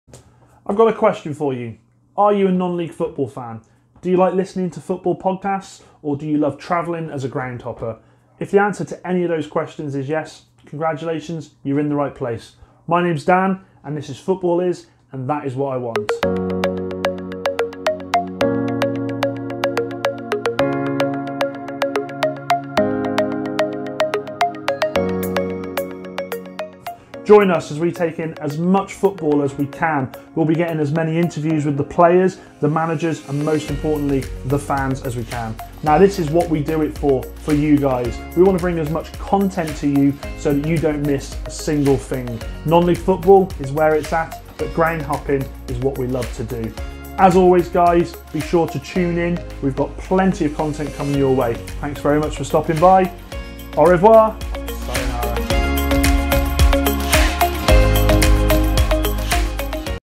Welcome New Viewers and Subscribers!
To all the new viewers and subscribers, I say a big welcome to you all!

Join us in our quest for Football!

You love Non League? - So do we!
You Love Groundhopping? - So do we!
You Love Football Podcasts? - Guess what... So do we!

We can't wait to share the highs and lows in the game we love!

Be sure to share us with your friends!